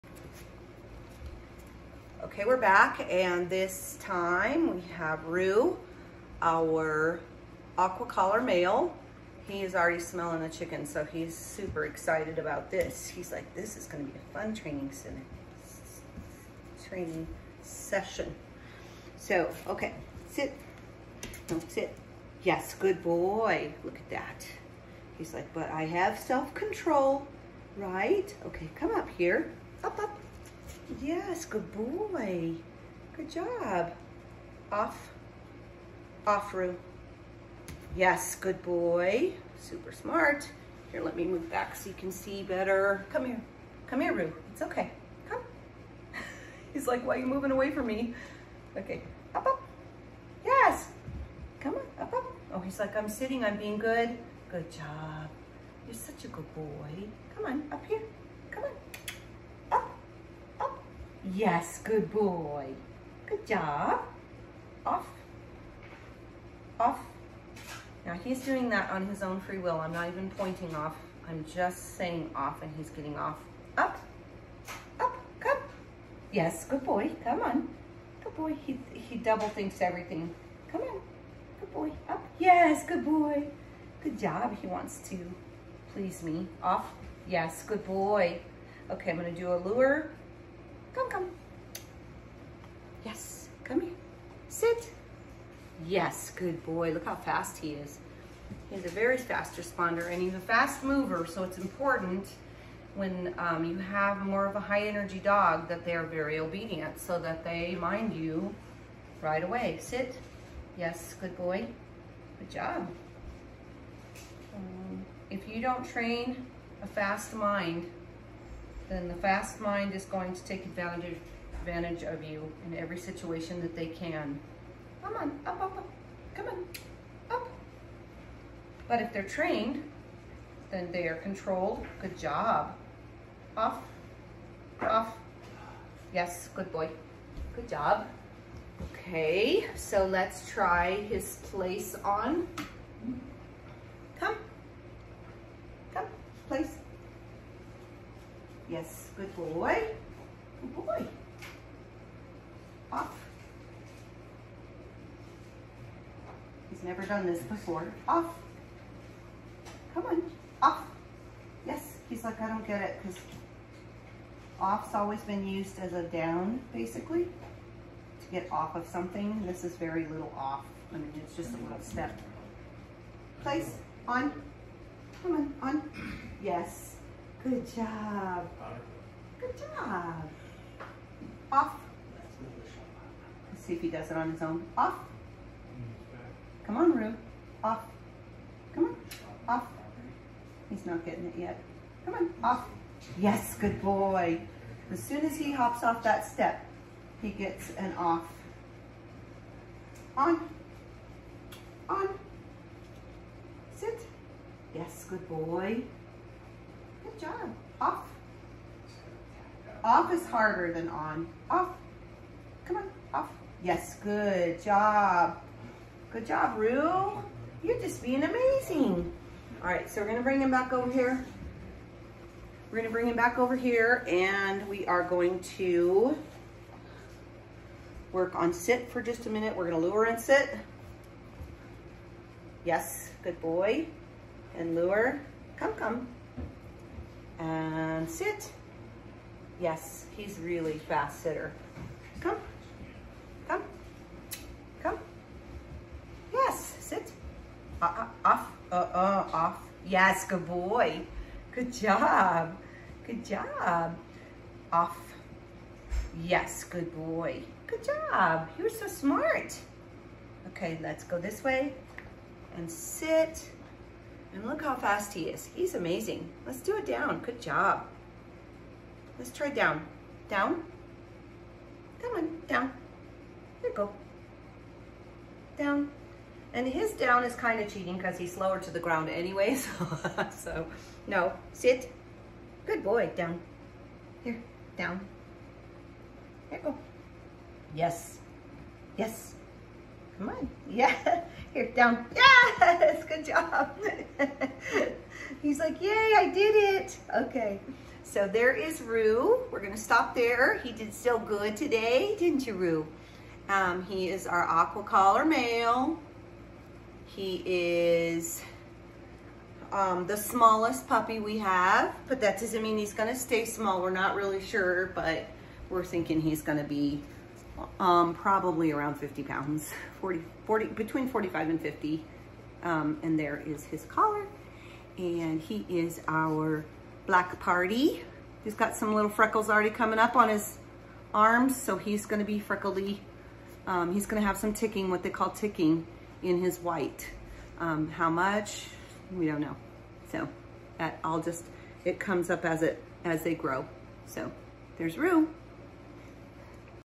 Standard Poodle Puppy Training, Roo- ON and OFF game
Games we play with puppies to help them learn English, The on and off or up and off game is one of our earliest games because it is easy at this age to get a puppy to climb up on you! If you can get them to climb up, you can easily teach them ON and Off which will be used widely when they are adults.